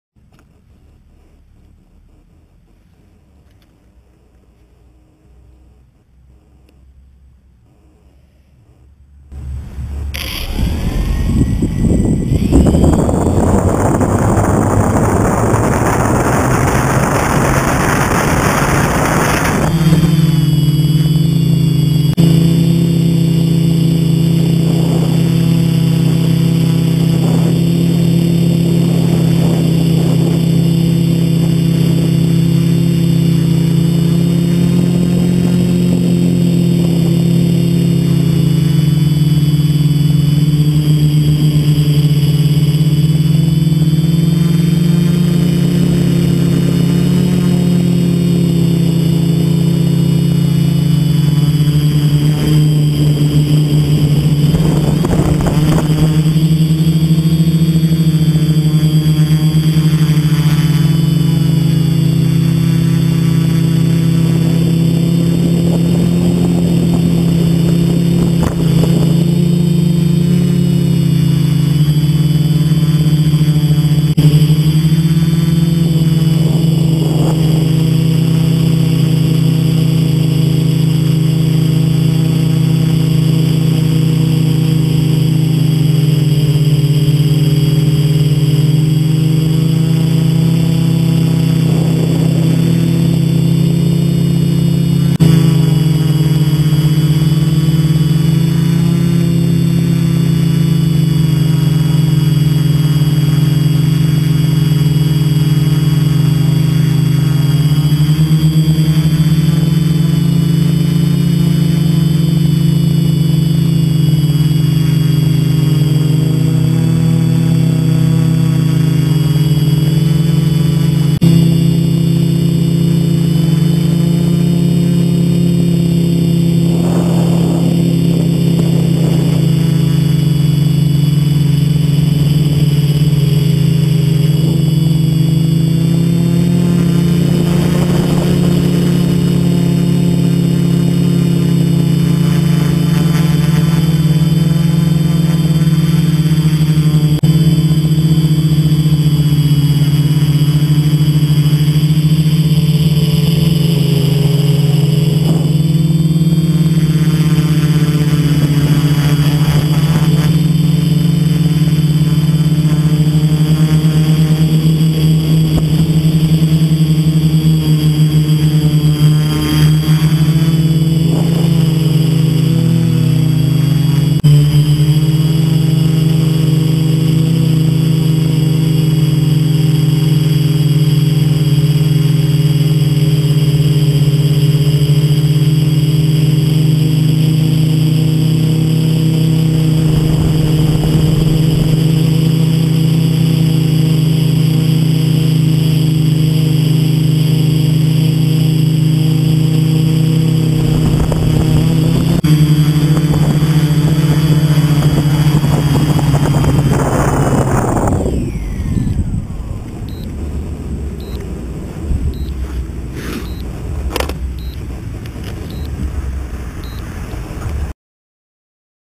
Gimbal BLDC test
Gimbal BLDC prototyp, domyślne ustawienia. Przewody od czujnika utrudniają pracę...